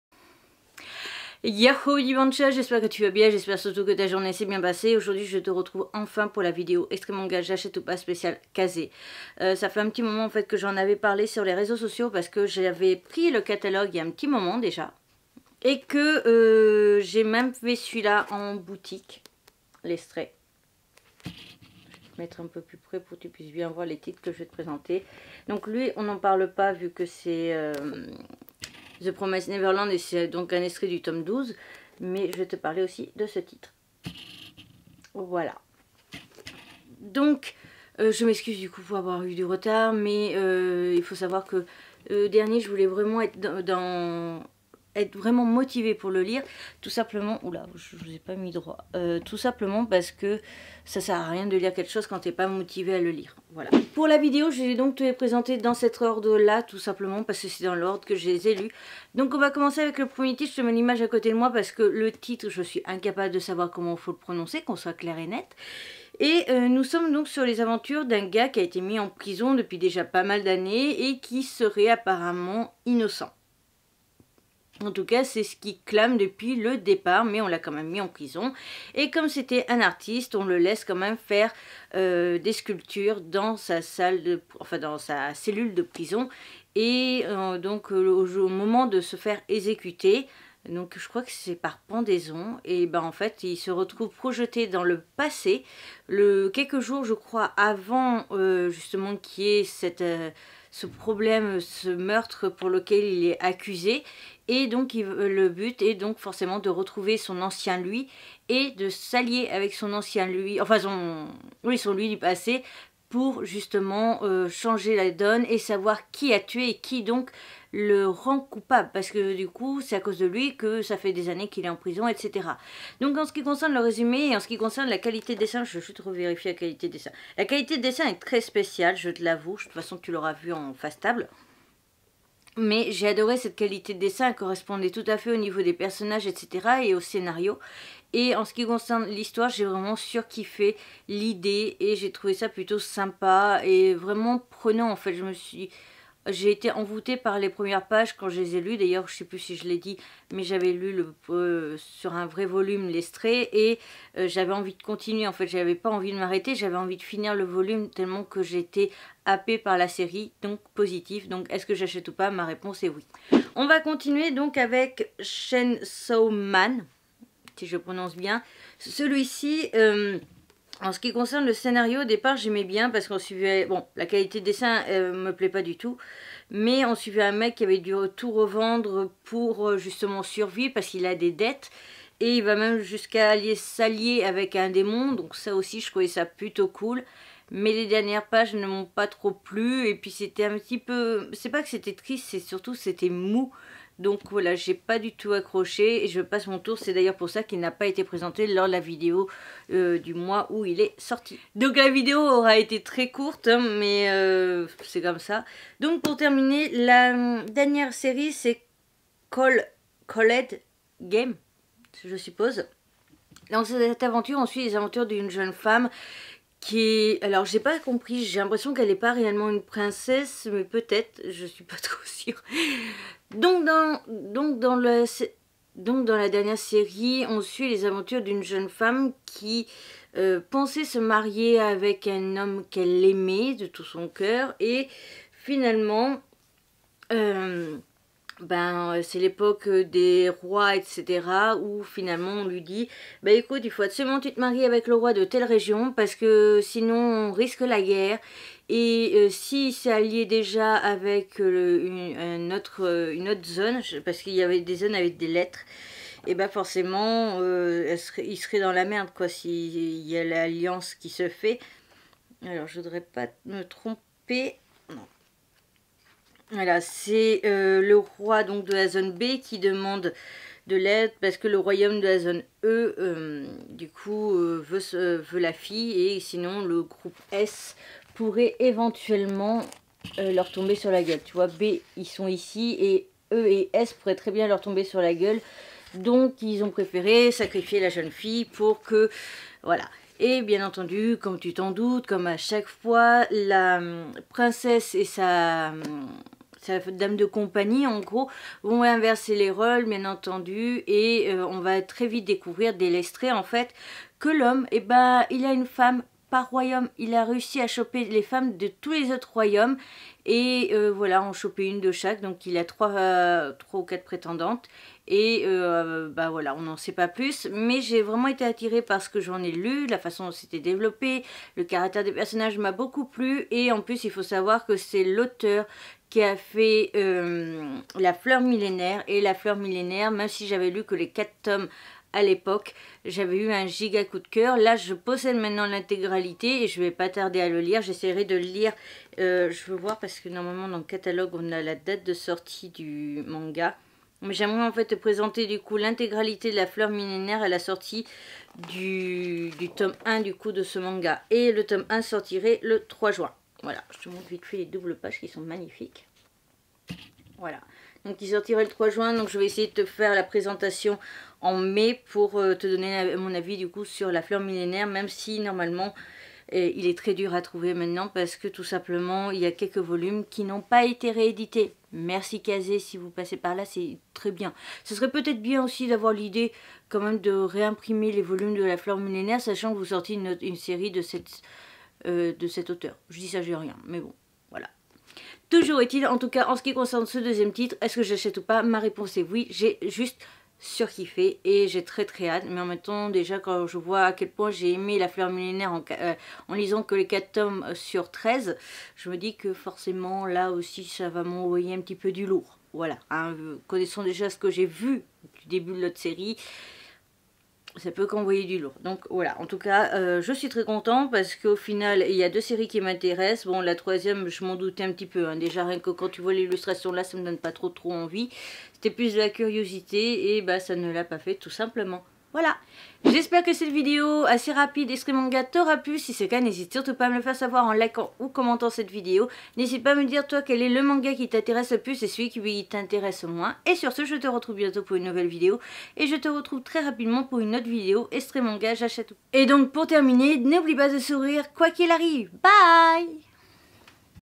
#22 Extrait manga : Called Game/Chainsaw Man/Doppelgänger/
Résumé called game :
La princesse Alna est promise au roi d’un pays voisin dans le cadre d’une alliance politique. Sur les conseils de son père, elle échange son identité avec celle de sa servante, Camilla, en arrivant chez son futur époux ! Là, elle découvre une cour déchirée par les intrigues et cinq autres prétendantes au trône, qui désirent accéder au pouvoir plus que tout… Pour survivre et espérer rentrer chez elle un jour, il lui faudra garder le secret de son identité !

Doppelgänger Résumé
Accusé à tort dans l’affaire des meurtres en série de sculpteurs perpétrés le long de la ligne Yokohama, Makoto Kenzaki est exécuté. Par un coup du sort, céleste ou diabolique, il se retrouve projeté dans le temps juste avant que les crimes ne soient commis ! Pour empêcher les meurtres à venir et se venger du véritable assassin, le Makoto Kenzaki du futur choisit comme partenaire… son double du passé ! “Kenzaki du futur” et “Kenzaki du passé” vont alors se liguer pour enrayer l’affaire et poursuivre le coupable de cette énigme criminelle à suspense. L’enquête des deux alter ego contre le destin débute maintenant !

Résumé Chainsaw Man :
Pour rembourser ses dettes, Denji, jeune homme dans la dèche la plus totale, est exploité en tant que Devil Hunter avec son chien-démon-tronçonneuse, “Pochita”. Mais suite à une cruelle trahison, il voit enfin une possibilité de se tirer des bas-fonds où il croupit ! Devenu surpuissant après sa fusion avec Pochita, Denji est recruté par une organisation et part à la chasse aux démons…